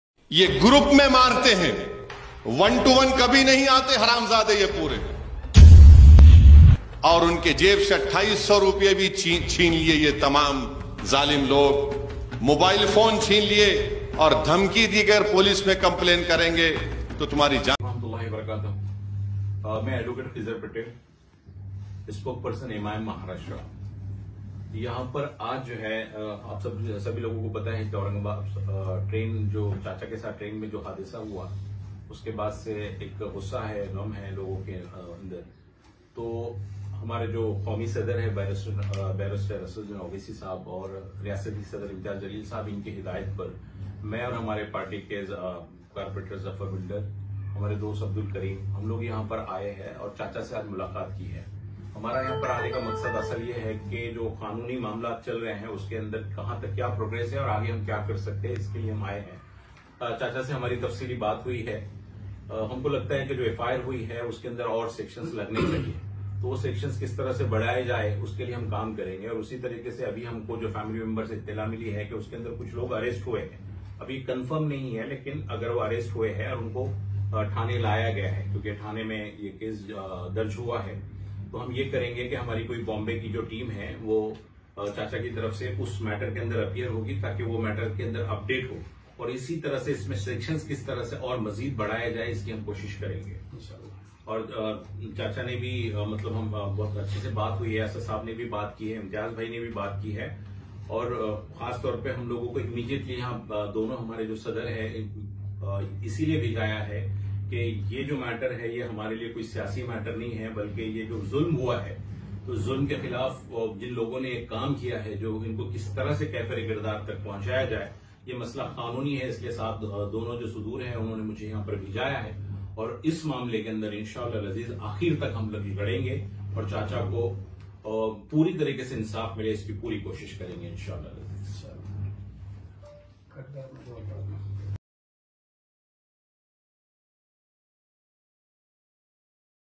महाराष्ट्र में मुसलमान बुज़ुर्ग के साथ मारपीट करने वाले पुलिस में भर्ती होने के लिए जा रहे थे!
हैरत की बात ये है कि महाराष्ट्र में मुसलमान बुज़ुर्ग के साथ मारपीट करने वाले पुलिस में भर्ती होने के लिए इम्तिहान देने जा रहे थे, मुसलमानों पर ज़ुल्म करने वाले गुंडे हमेशा झुंड में ही आते हैं